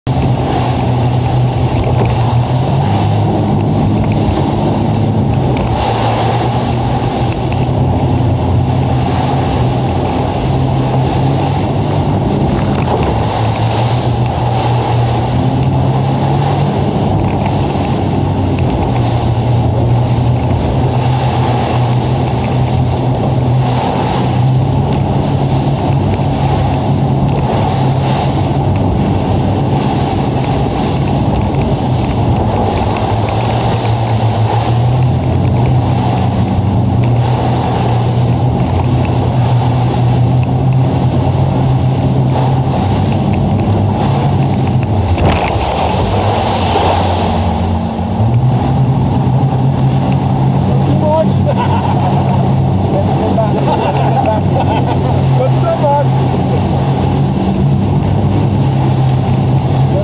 60 sec Magellanstrasse 360 Clip
End of the World Expedition, Puerto Montt - Cape Horn with Rigid Inflatable Boats